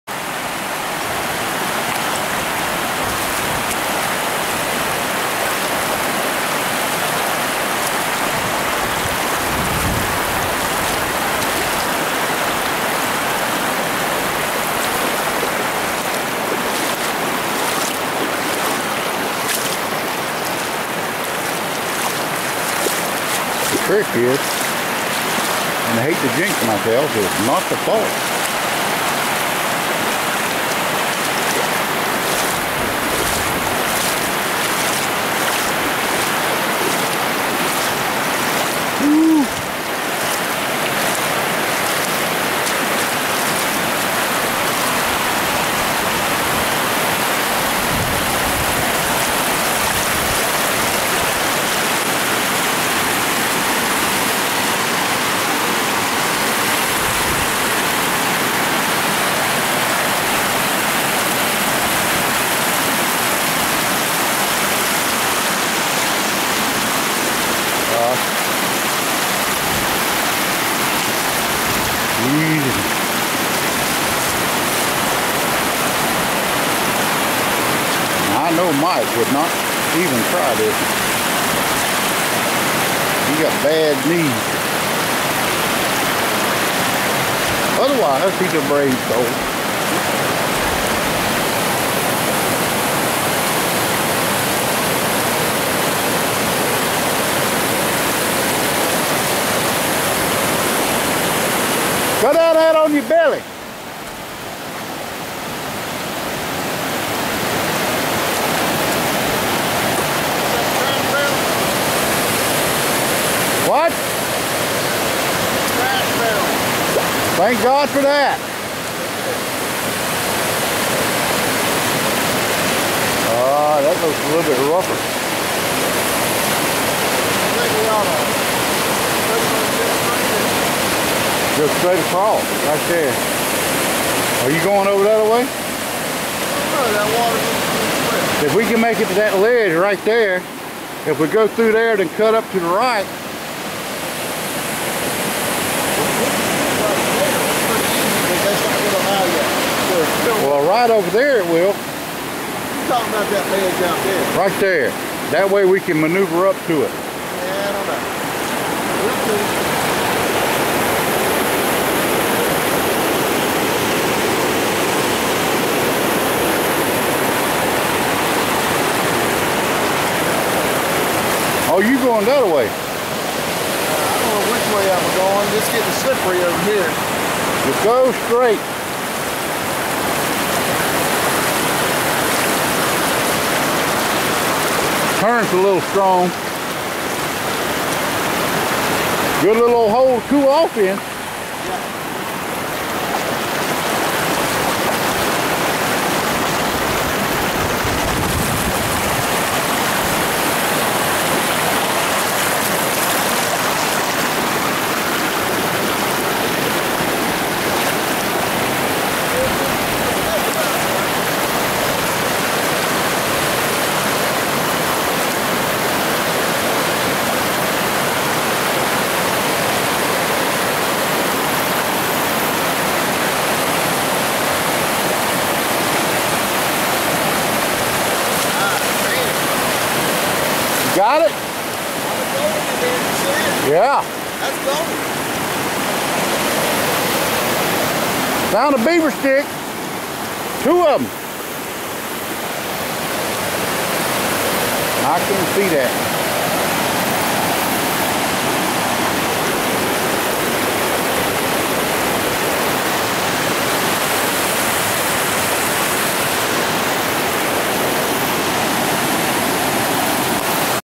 Ott Shoals and Old Dam, North Tyger River
What a beautiful place. Hard to find and hard to get to. well worth the effort.